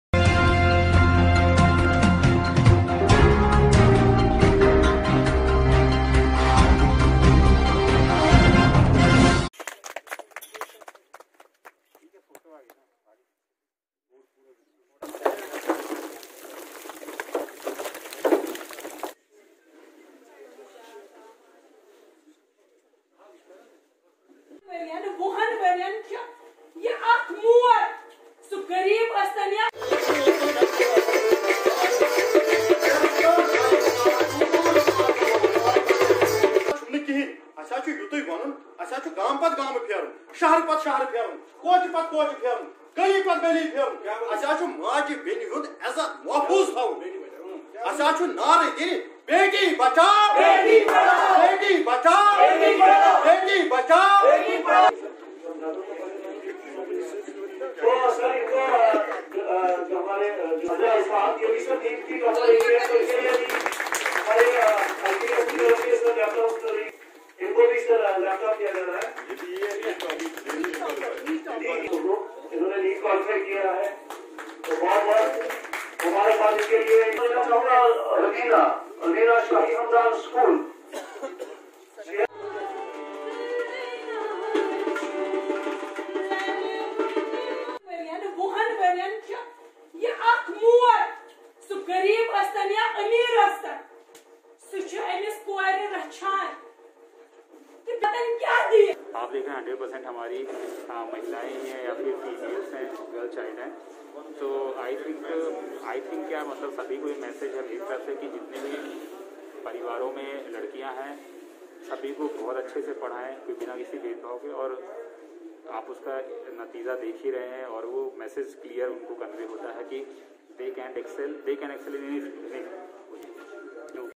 Degree College Shopian On the occation appreciation was highlighted for the women. JKNO
International Women's Day Celebrated in Hilly District Shopian.
Wani nazir
Women's International Day was organised today on 8th March 2021 by the District Administration Shopian in collaboration with the District Programme Officer ICDS Shopian at Govt. Degree College Shopian.

 achieved various leadership roles in various fields..During the programme NEET/JEE 2020 girls who qualified were felicitated by distribution of Laptops among the 09 qualifiers, besides the 10th academic girl toppers and Sports women/girls were also felicitated on the occasion by the Worthy District Development Commissioner Shopian .
The Worthy District Development Commissioner Shopian enlighted the role of women in the society..
The programme was attended by all the District Officers, BDC Members, Supervisors ICDS,Officials from ICPS, Civil Society Representatives, the artists from the drama and song division who make the programme colourfull by their performance on the theme of "Celebration of International Women's Day -Lets All choose to challenge" and Beti Bachao Beti Padhao..

Deputy commissioner Shopian, Sachin Kumar said the women in Kashmir have played a pivotal and significant role in the society. “The women who is daughter, sister, mother, are contributing selflessly to the society, he said  Prothe women in Kashmir have been making advances in almost all the fields, particularly academics and medicine. “In almost all the results of schools and colleges, the girl students are outshining the boys, he added.